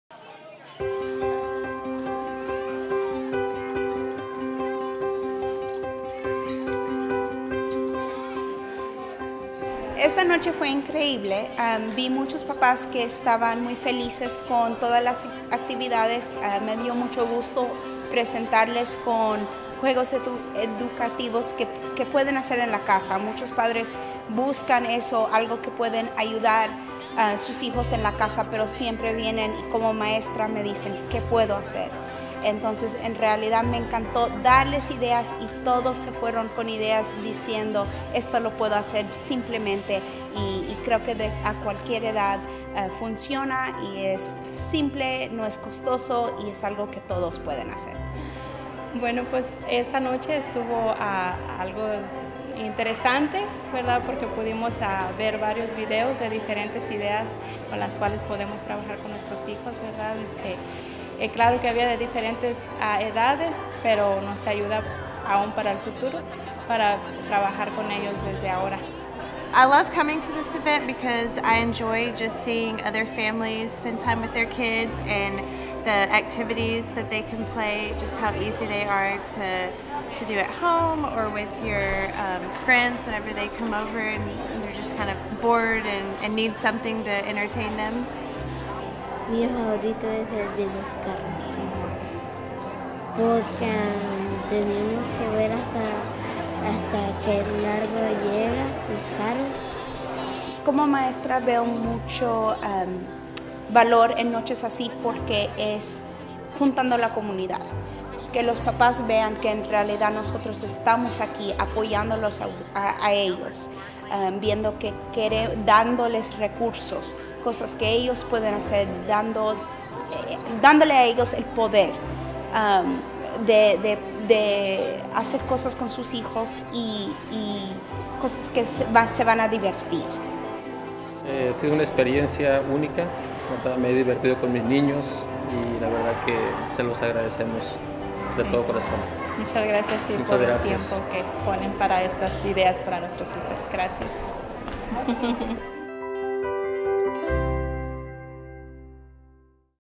ReadyRosie Family Events Video
ReadyRosie families describing their favorite ReadyRosie activities at a family event. In English and Spanish.